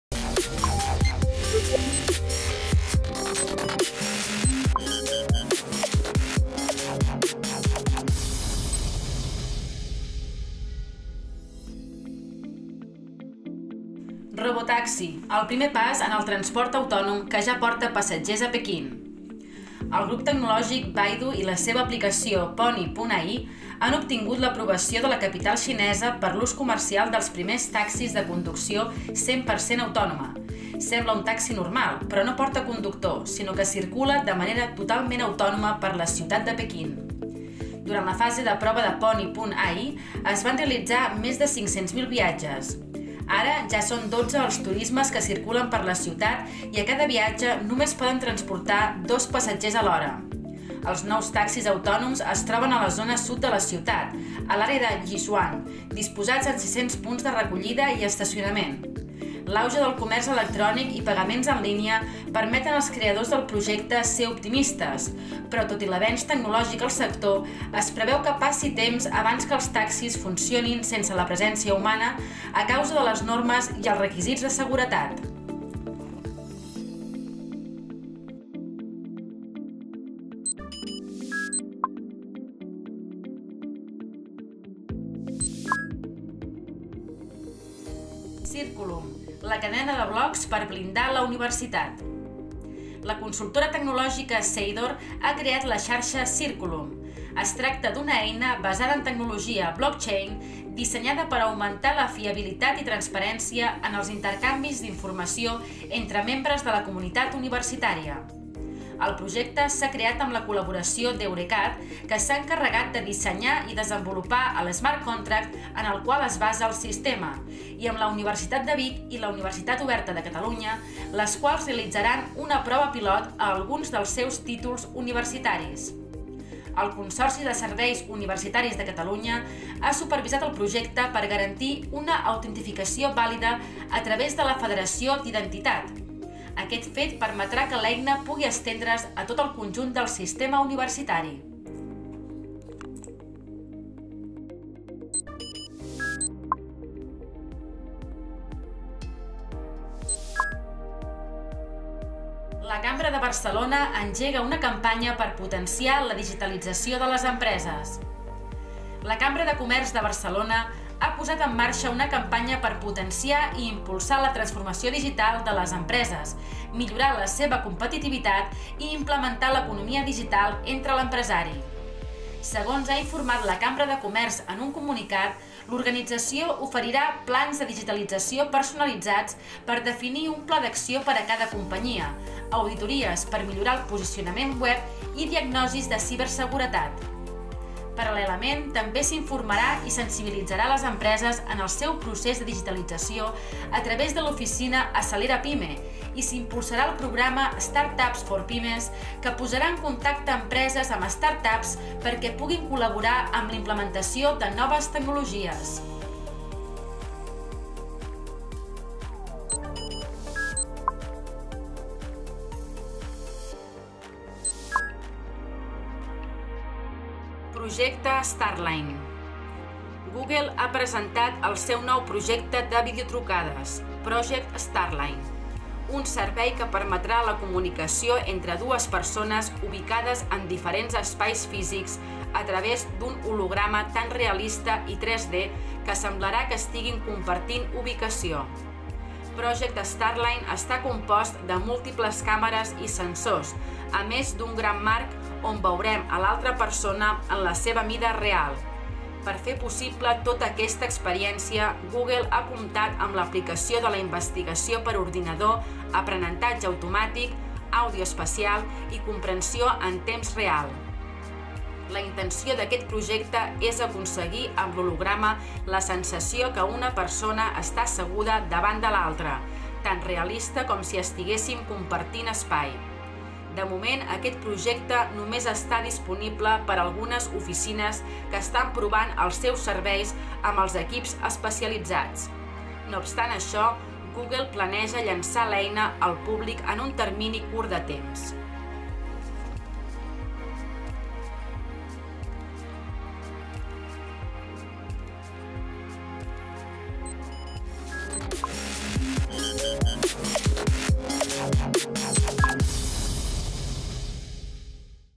Infotech 09/12/2021
Robotaxi, el primer pas en el transport autònom
Circulum, la cadena de blocs per la universitat
La cambra de Barcelona i la digitalització
Project Starline